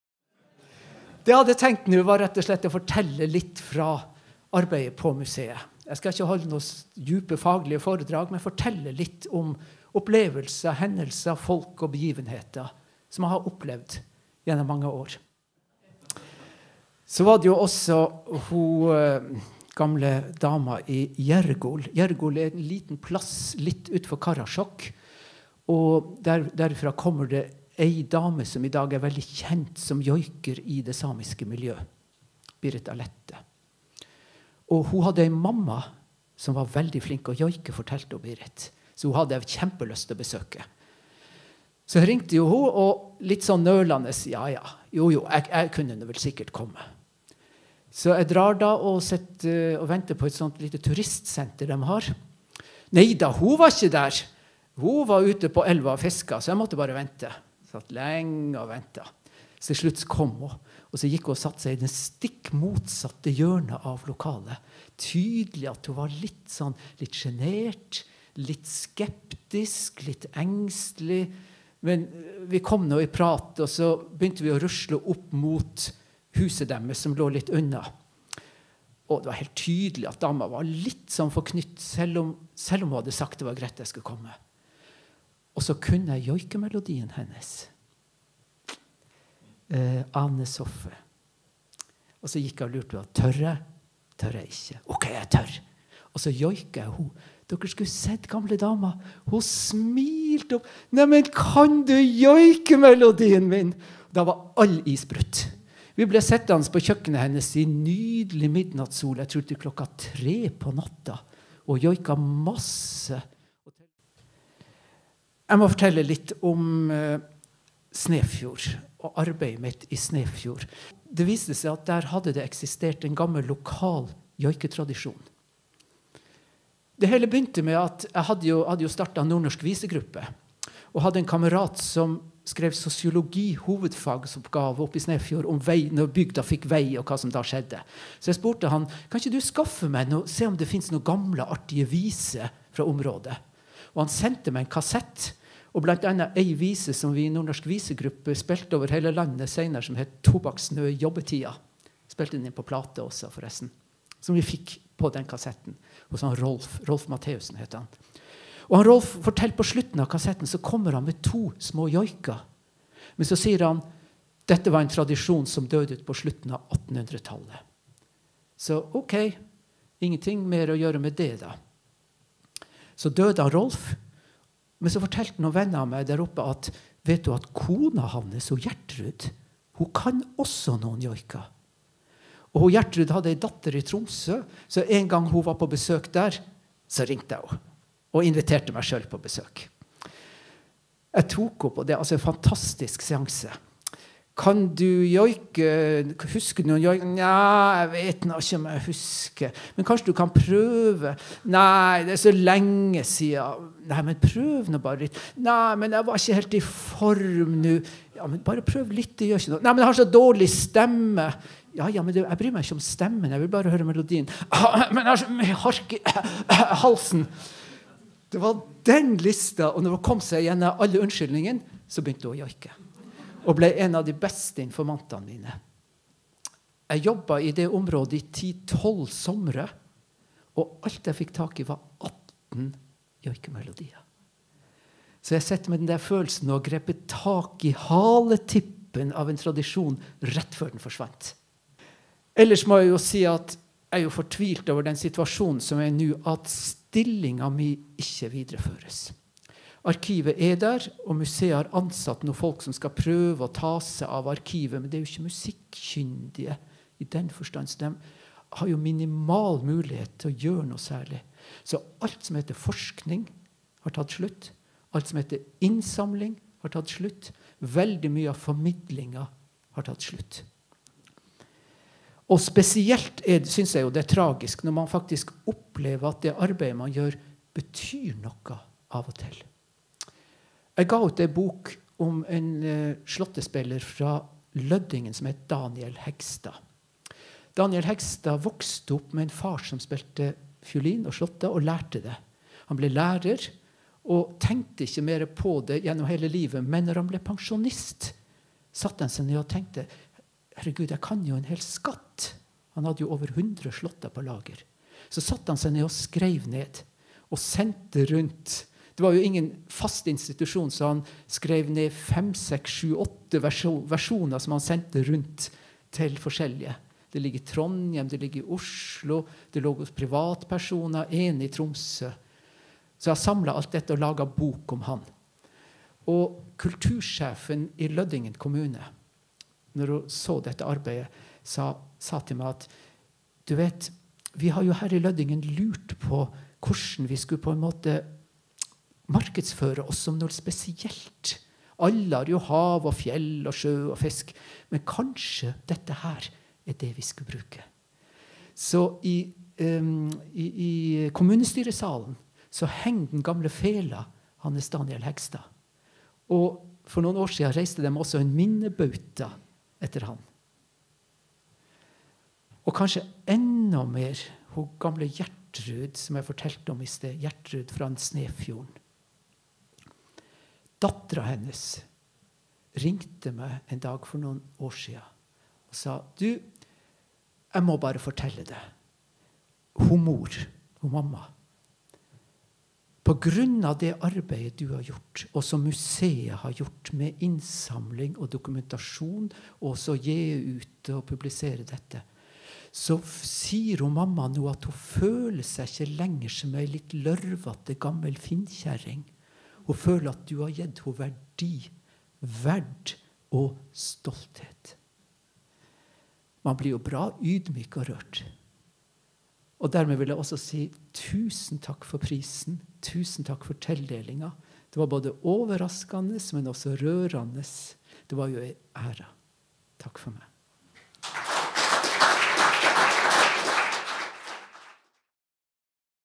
Prisen for folkemusikk og folkedans 2022 - Ola Graff
Hvert år deler Norsk senter for folkemusikk og folkedans ut Prisen for folkemusikk og folkedans til personer som har gjort en ekstraordinær innsats på folkemusikk- og/eller folkedansfeltet.

I 2022 gikk prisen til Ola Graff på grunn av hans formidable arbeid over mange år. Videoen viser et utdrag fra talen Ola Graff ga under prisutdelingen på Riksscenen.